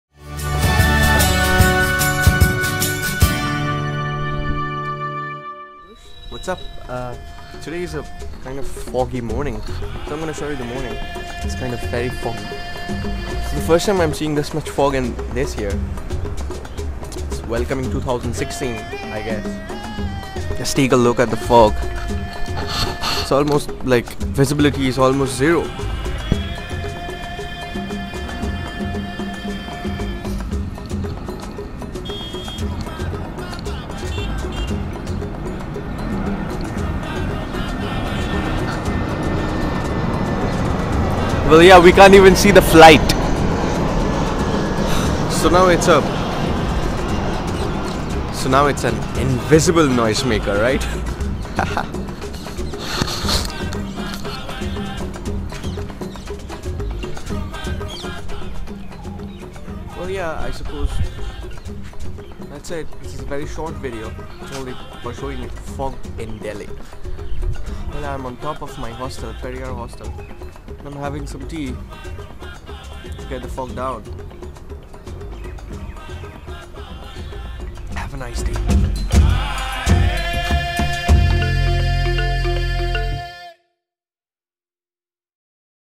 JNU.Delhi Fog from Periyar Top.
LifeBlog 33: The first heavy morning fog in Delhi this year, 2016.